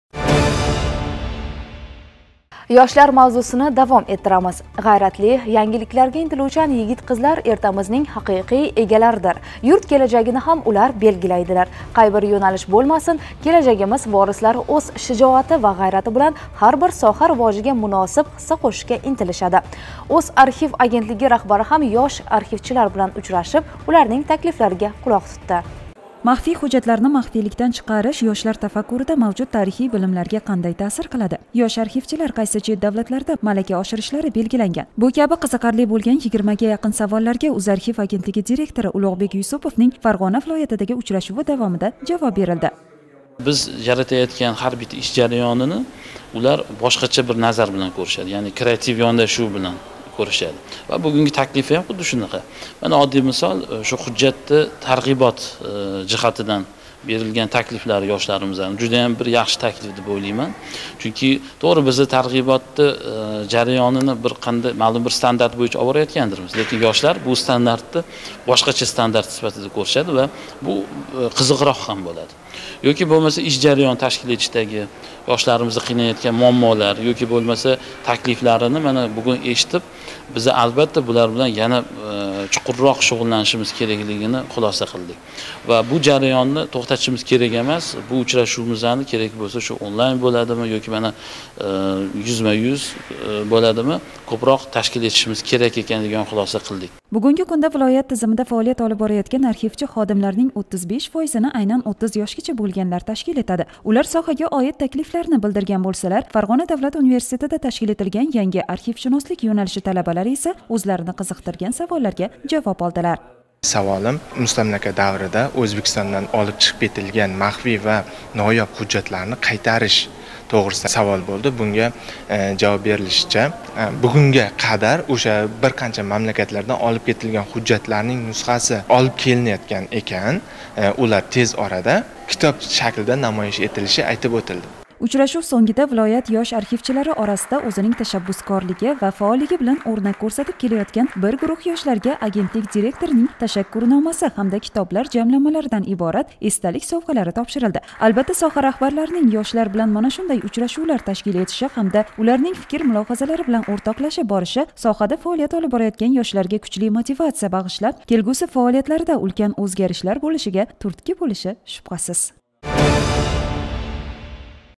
Ёшлар билан учрашув нима беради? Бу хақда "Ўзархив" агентлиги Улуғбек Юсуповнинг интервьюсида
ОАВ биз хақимизда

Фарғоналик ёшлар билан учрашувда  ёшлар муаммоларини ҳал қилиш, бўш вақтини мазмунли ташкил этиш бўйича очиқ мулоқот бўлди.

"Ўзархив" агентлиги директори Улуғбек Юсупов ўз интервьюсида ёшлар билан учрашувлар, очиқ мулоқотлар нима бериши хақида гапирди.

"Ўзархив" агентлиги Ахборот хизмати